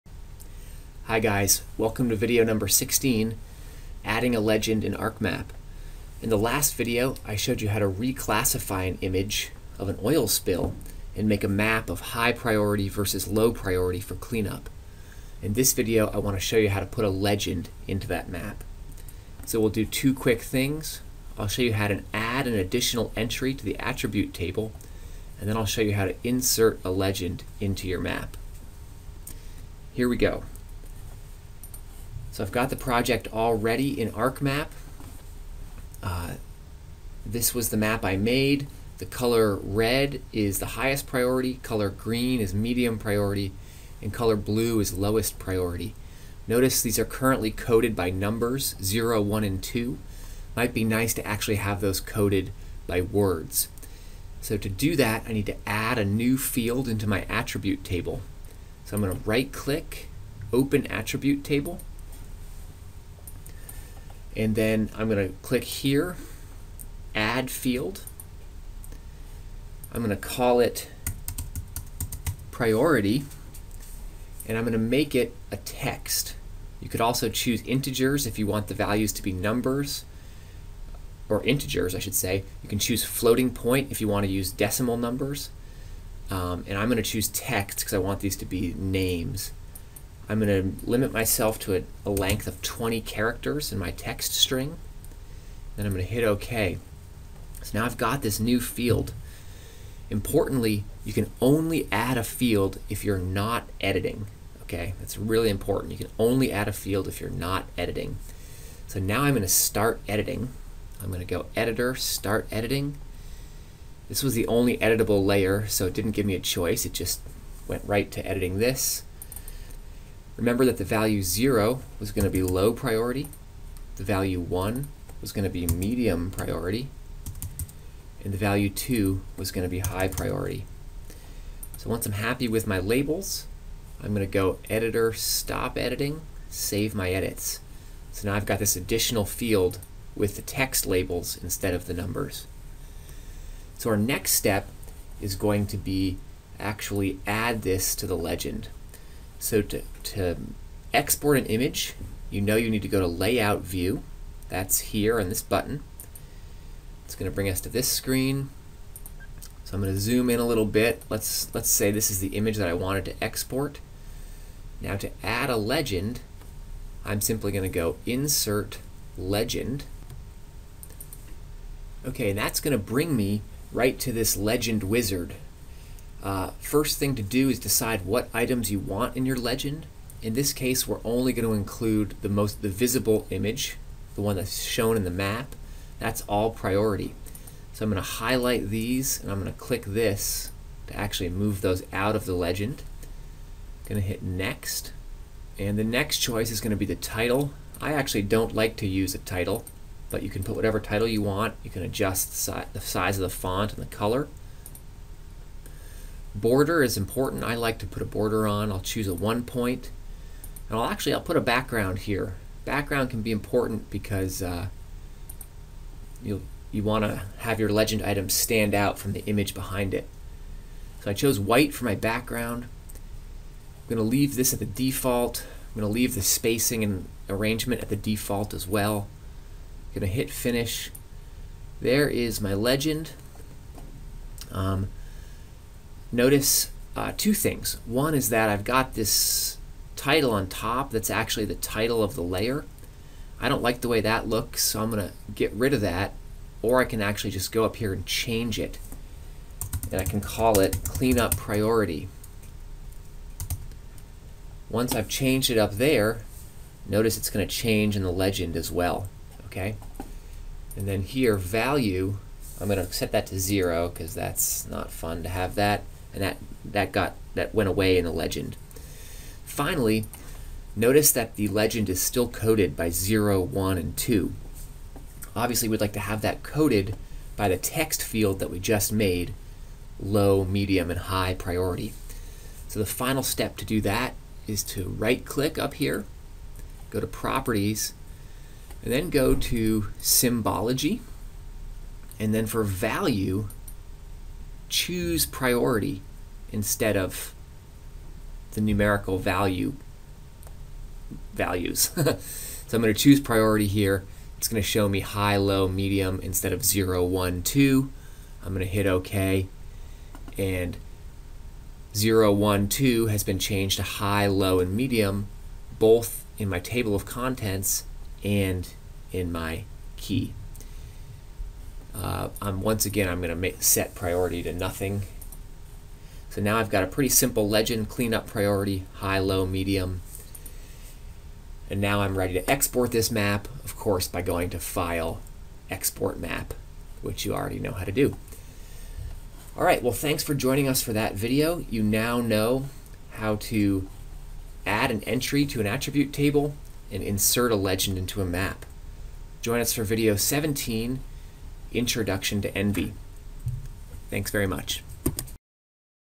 v16 adding a legend in ArcMap
Adding a field to the attribute table @ 0:45
Inserting a legend @ 2:25
Modifying a legend @ 3:55